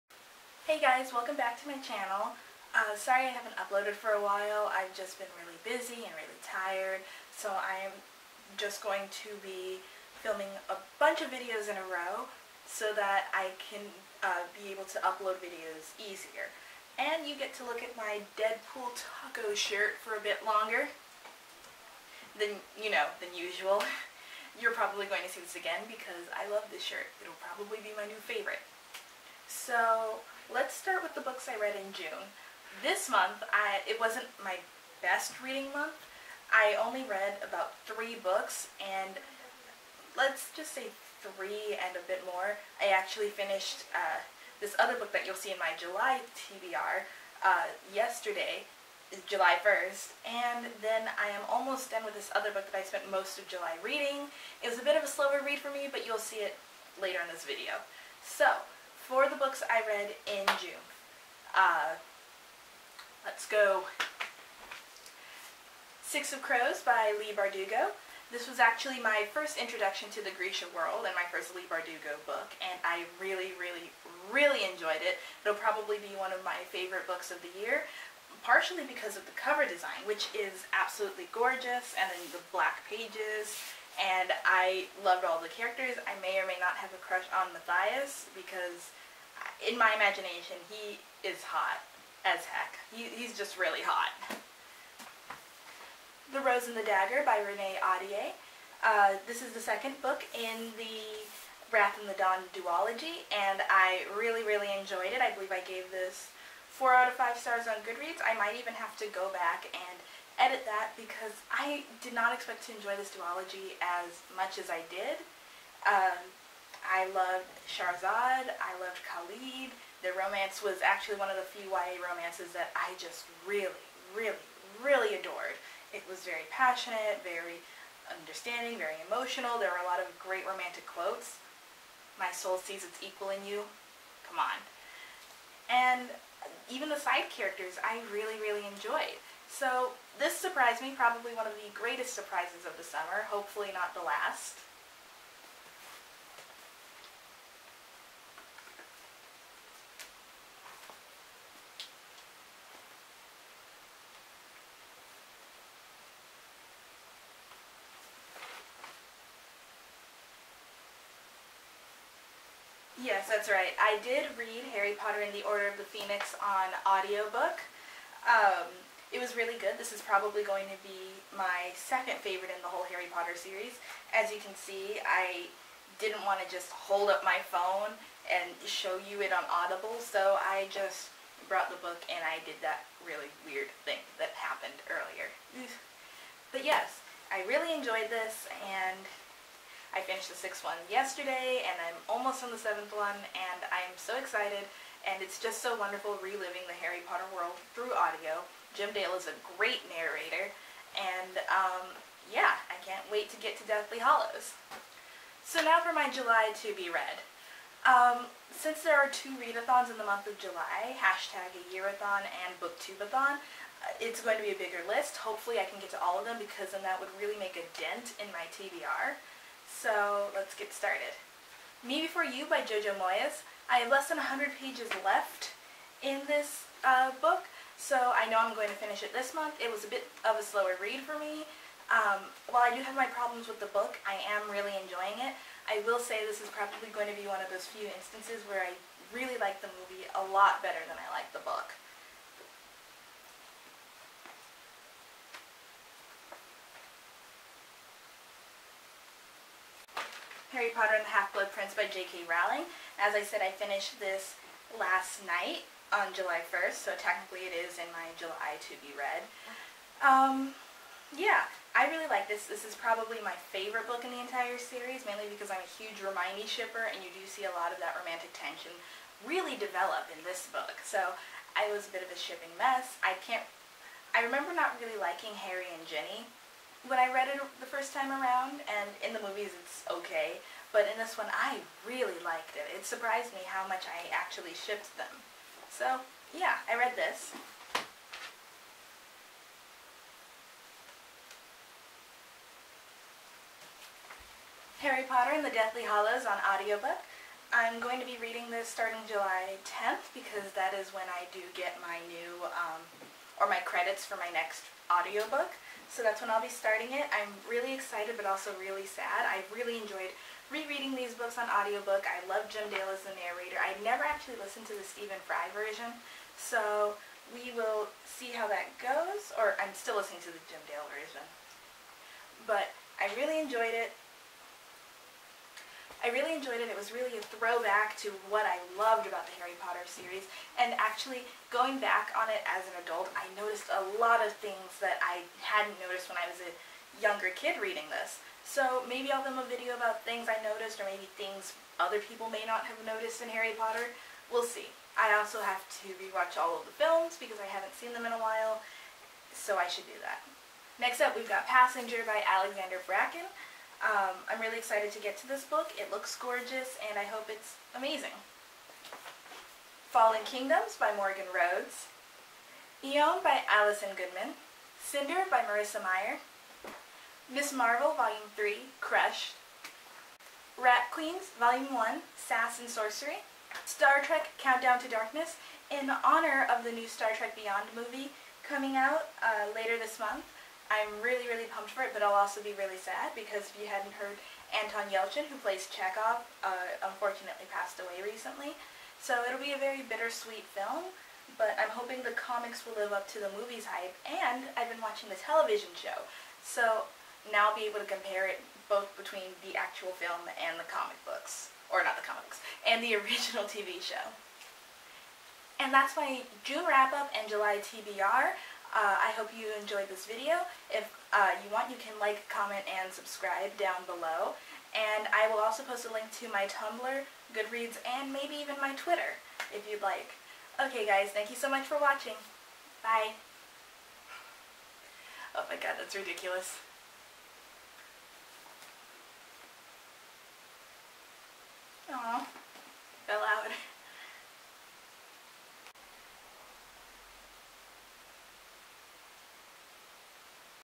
June Wrap Up and July TBR
It wasn't a great month for reading in June, but let's hope July is even better!